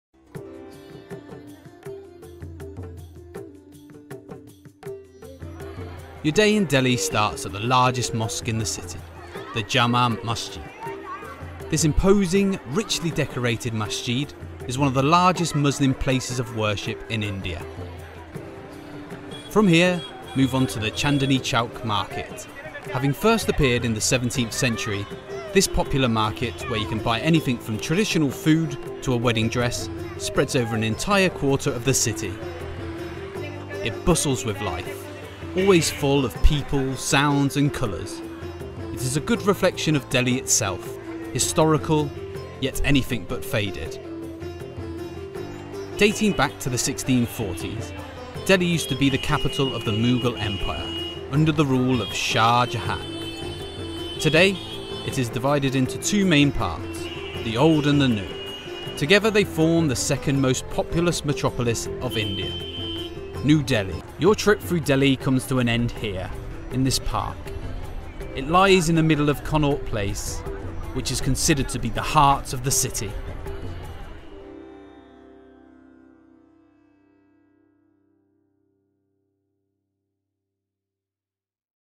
One day in New Delhi (Trailer): 360° Virtual Tour with Voice Over
The imposing Indian architecture will definitely catch your eye in the capital city of India - New Delhi. Though its history only dates back to the beginning of the 20th century, the city has a lot to offer. During the day or all lit up at night, its palaces and temples will surely take your breath away. The colourful bazaars are a great opportunity for buying local products and tasting local food. Explore the above mentioned and much more in this 360 video.

Find out more about New Delhi on Sygic Travel Trip Planner with detailed info and map